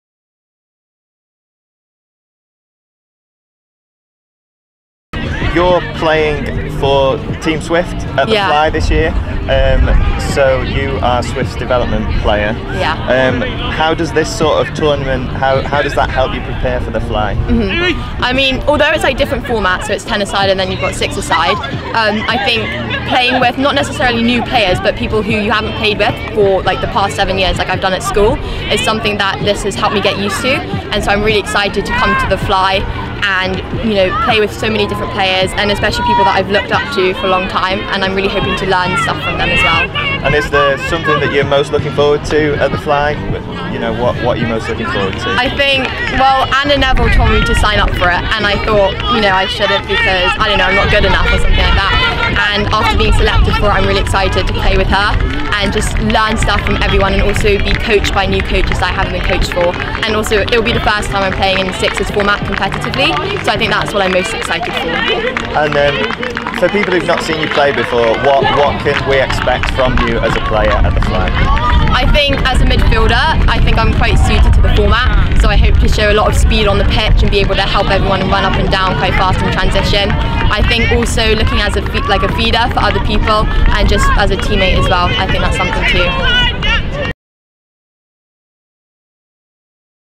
"I think I'm quite suited to the format" | Swift's development player, Emma Pearce
Swift's development player, Emma Pearce, speaks to us about all things The Fly after starring at Home Internationals 2022 for England Junior Women's (Red).